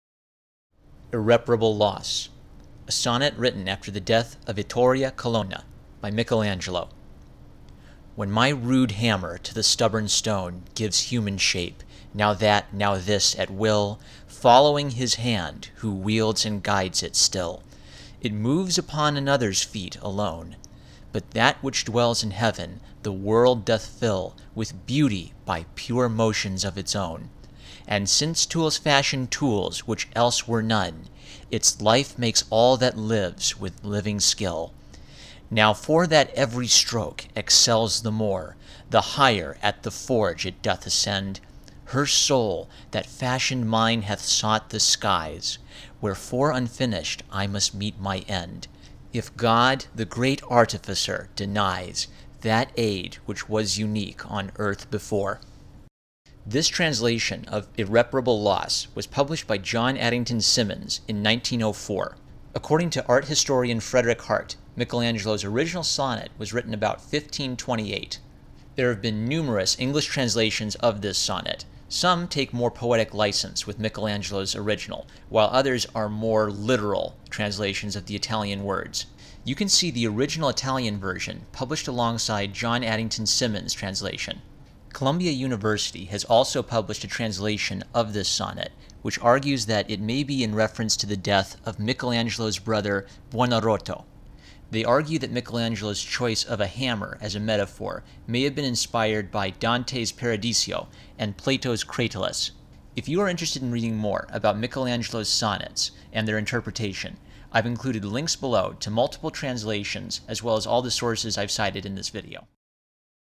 Michelangelo's Irreparable Loss Gives Insight to his View of Sculpture
Published as Sonnet LXI "Irreparable Loss" in John Addington Symond's more poetic translation

Art historian Frederick Hartt discusses this sonnet in relation to Michelangelo's sculpture in his introduction to "The Complete Sculptures of Michelangelo."

A more literal translation and discussion of this sonnet published by Columbia University

I found the portrait of Vittoria Colonna via Artsy.net's article on her relationship to Michelangelo

The thumbnail art is my own personal study of the hand of Michelangelo's David from the plaster cast at the Victoria and Albert Museum in London.